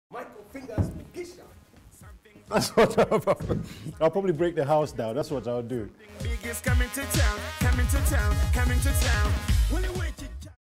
Here is how 'Micheal Fingers' would react if he won the ‪#‎BillionLotto‬ jackpot;
Billion Lotto - Changing Lives - Uganda National Lottery